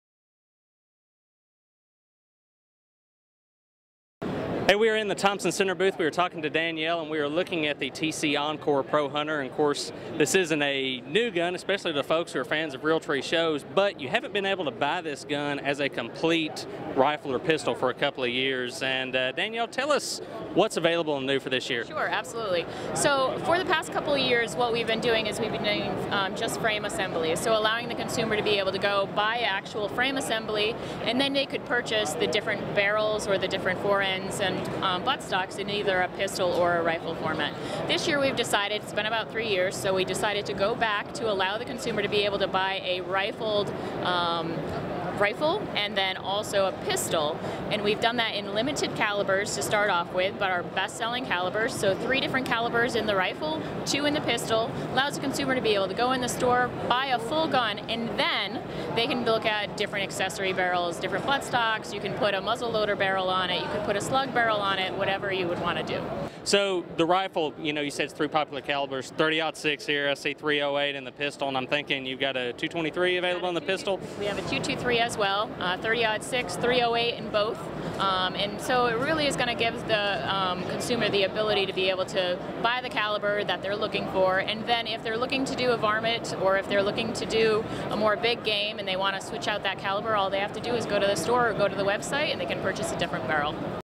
New T/C Encore Pro-Hunter Rifle and Pistol Packages
Thompson / Center has reintroduced the Pro Hunter rifle and pistol as complete firearms in a variety of calibers